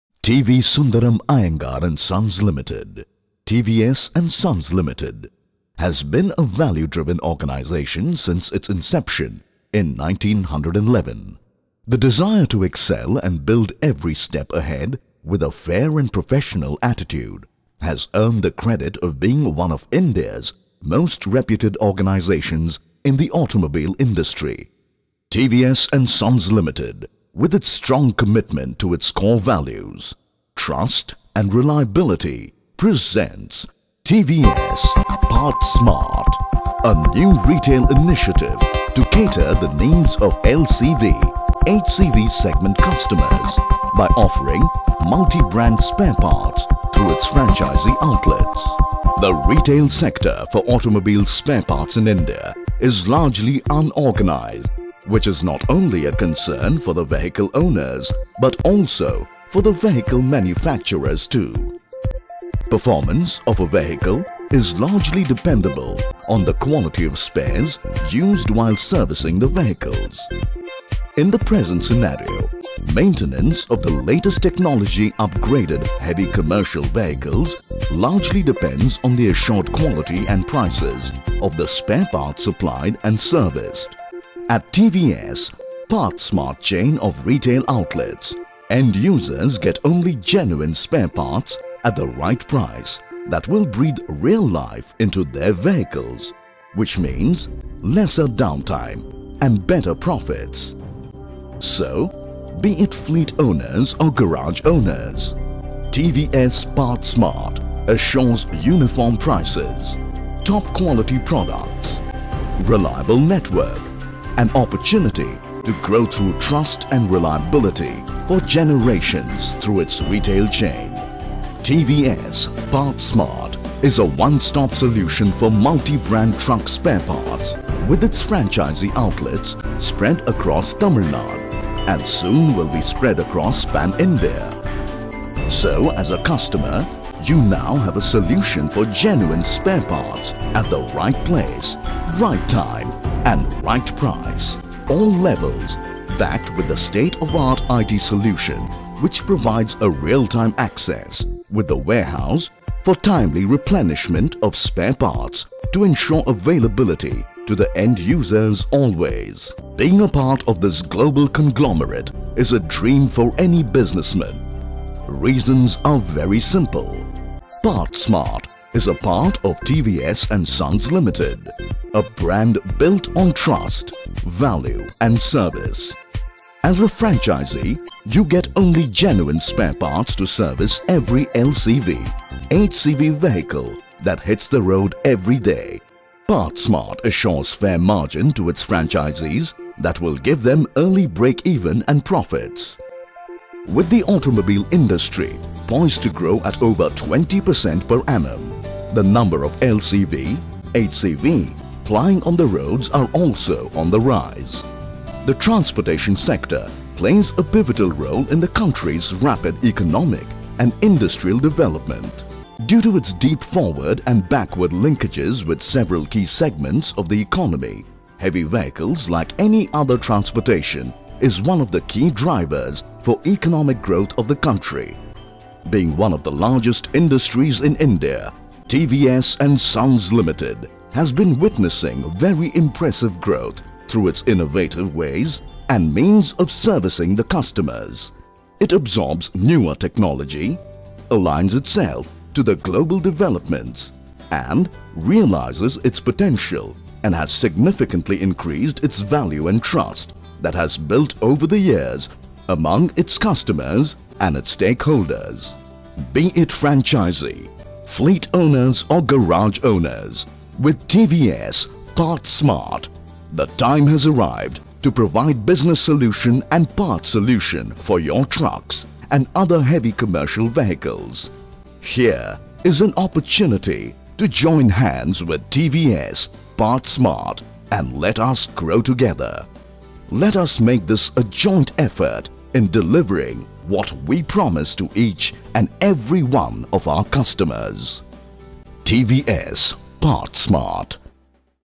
TVS PartsMart [Corporate Film]
Promotional video done for TVS about their new brand PARTSMART